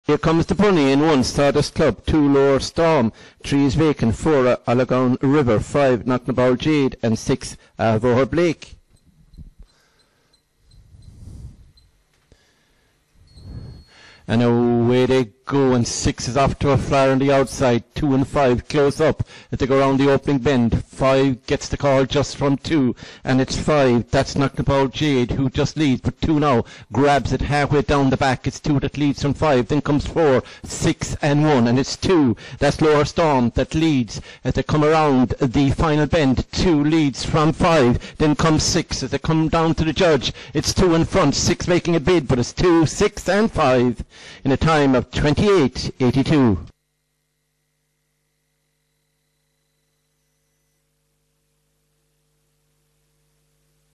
IGB - Follow Us On Facebook 04/08/2018 Race 6 - Tralee
Track Name: Tralee
Date: 04/08/2018
Hour: 20:59

Results

1st    2     LOHER STORM         6/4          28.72
2nd    6     AHAVOHER BLAKE      2/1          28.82
3rd    5     KNOCKNABOUL JADE    1/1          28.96
4th    4     ALLAGHAUN RIVER     2/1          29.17
5th    1     SAWDUST CLUB        5/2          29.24

|TRL_2018_08_04_06